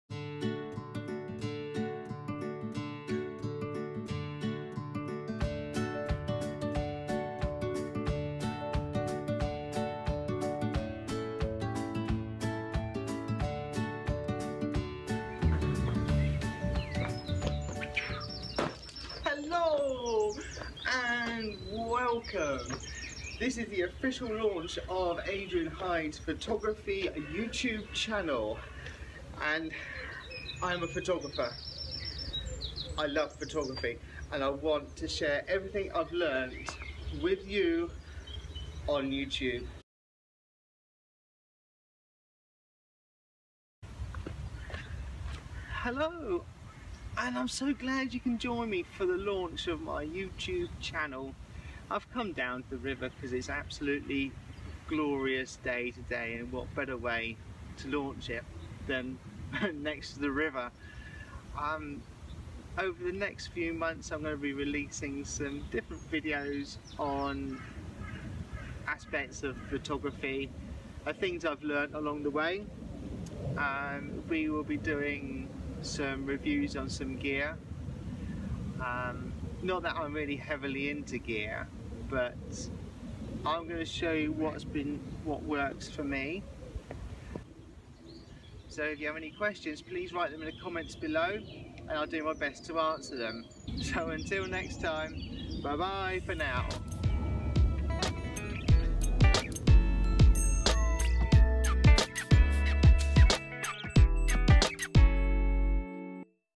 Adrian Hyde Photography YouTube launch video
This is the YouTube launch video for Adrian Hyde Photography. I will be posting videos on the subject of photography.